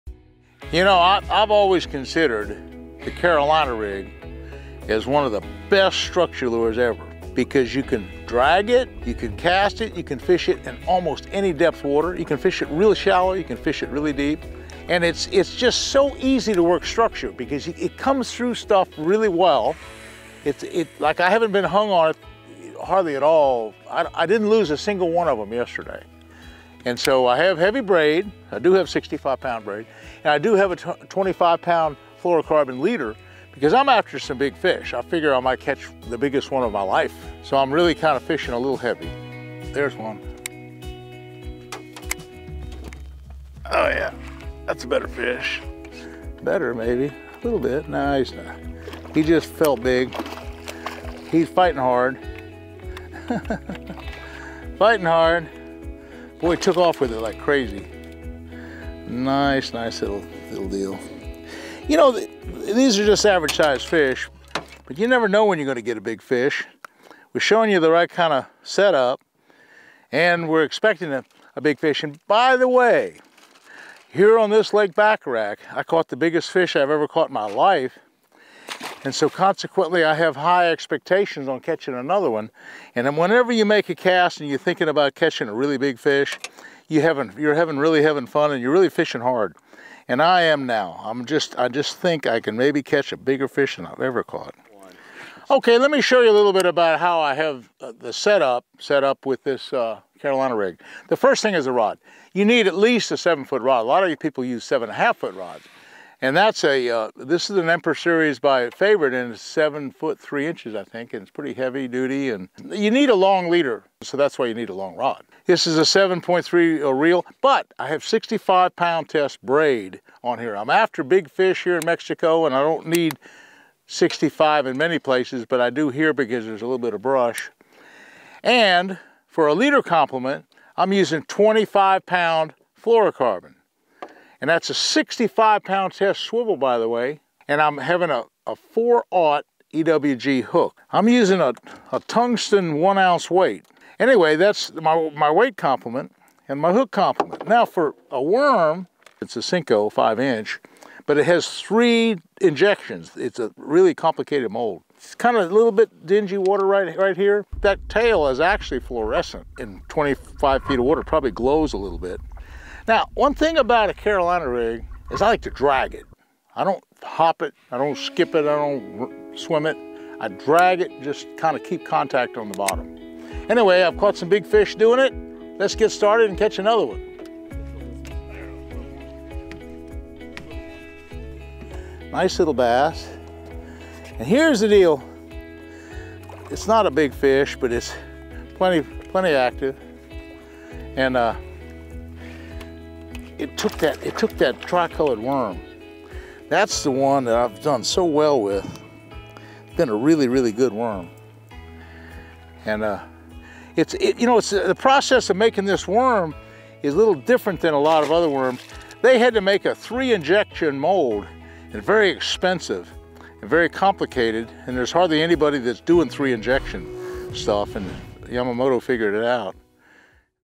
The Basics of Carolina Rig Fishing with Roland Martin
Tackle innovation and new rigging methods make it easy to overcomplicate bass fishing, but the tried and true approaches remain excellent producers. The Carolina rig is a case in point. We caught up with legendary angler, Roland Martin for a lesson in common-sense Carolina rigging using a handful of simple tackle.

From terminal tackle to plastic, Roland walks us through how to rig the affordable setup. And contrary to popular opinion, light line and extreme stealth isn't always a must to put quality bass in the boat. Martin explains why he doesn't shy away from a heavy braided mainline in certain situations.

• Offset EWG Hooks, 4/0
• Tungsten Flipping Weight, 1-ounce
• Glass Bead, 8mm
• Barrel Swivel, 65lb
• Braided Mainline, 65lb
• Leader, 25lb Fluorocarbon